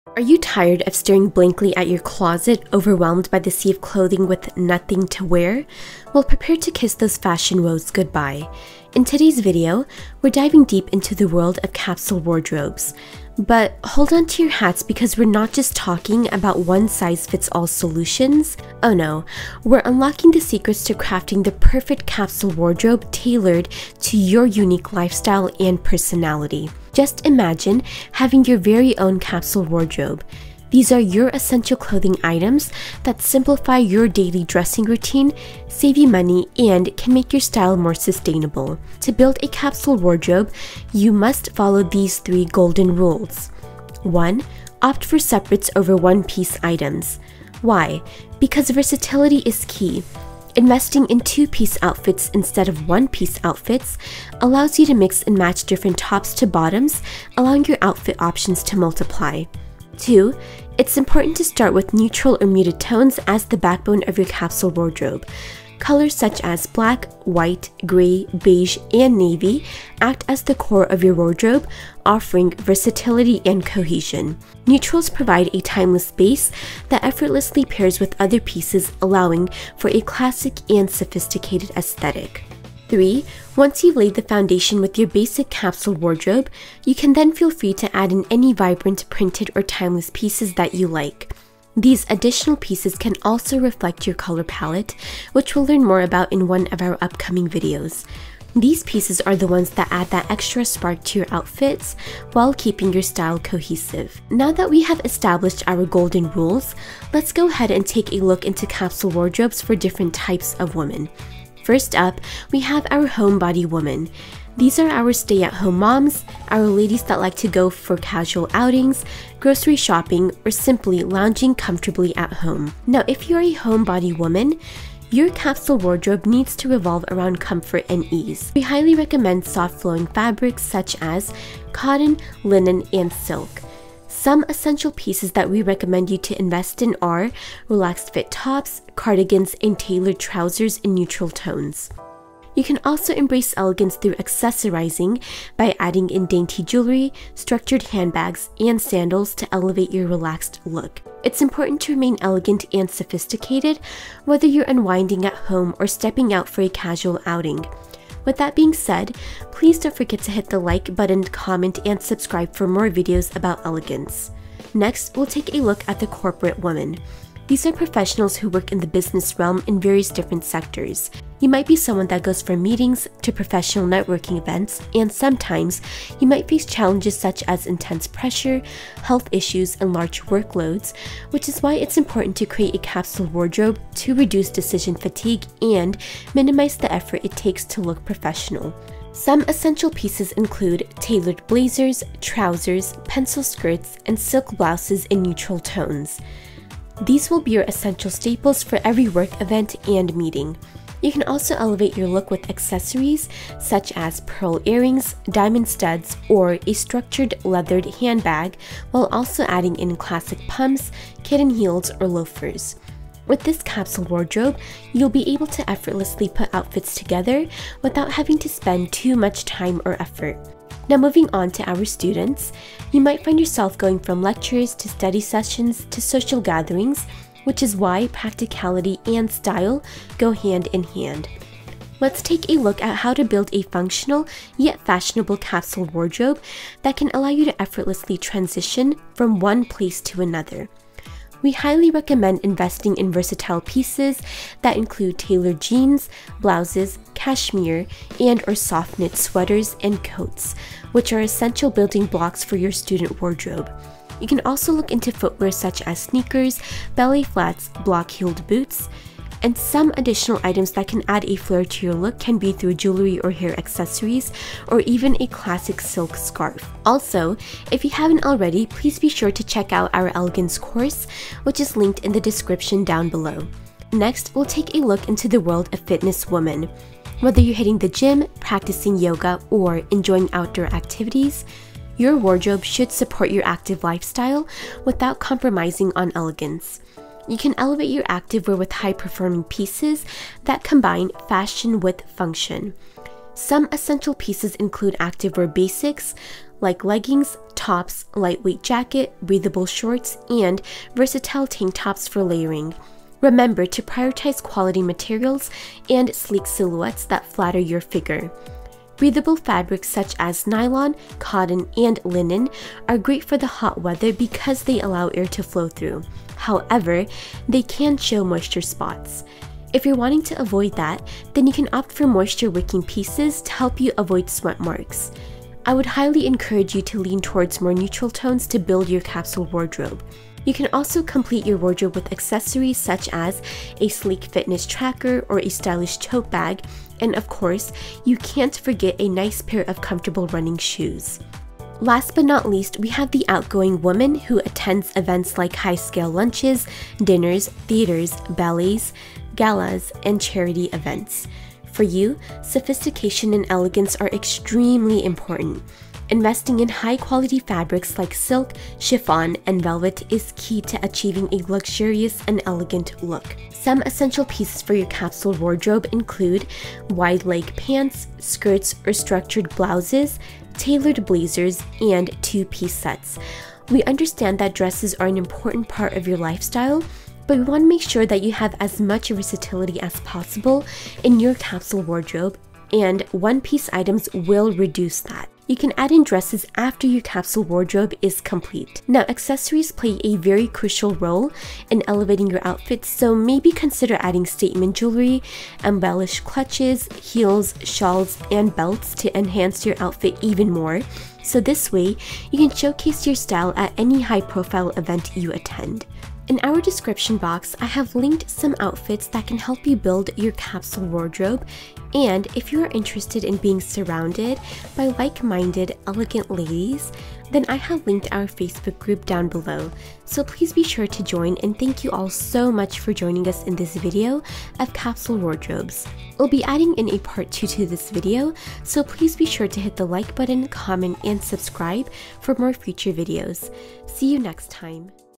Create your CUSTOM Capsule Wardrobe | 5 Wardrobes for 5 Types of Women and Lifestyles
In this video, we're diving deep into the world of capsule wardrobes, answering burning questions like "What are classic pieces for a capsule wardrobe?" and "What are the rules of a capsule wardrobe?"

Whether you're a fashion novice or a seasoned pro, we've got you covered with practical tips and expert advice on how to start a chic capsule wardrobe for beginners. Learn the essential rules of capsule wardrobe creation, from opting for versatile separates to building a foundation with timeless classics. Also, the video provides elegant outfit inspiration for various lifestyles and types of women.

Join us as we explore capsule wardrobes tailored to different lifestyles, showing you how to realistically look elegant and classy as a homebody/housewive/stay-at-home mom, corporate employee/businesswoman, university/high school student, active fitness enthusiast, or social and outgoing lady. Each wardrobe is carefully curated with essential pieces and accessories to suit every lifestyle and personality.

📌Outfit links

Amazon collection

Lightweight cotton cashmere
Fisherman crew sweater
Ultra-Stretch Ponte Straight Leg Pant
Baby Alpaca-Wool Cropped Cardigan
Washable Stretch Silk Notch Collar Blouse
100% Organic Cropped Cotton Polo Sweater
Bella Merino Sweater Polo Shirt
Slim-fit stretch cotton poplin shirt
Mia Satin Skirt

Chapters:
Intro (0:00)
Capsule wardrobe rules (0:41)
Homebody wardrobe (1:52)
Corporate woman wardrobe (2:47)
Student wardrobe (3:41)
Fitness woman wardrobe (4:36)
Outgoing woman wardrobe (5:43)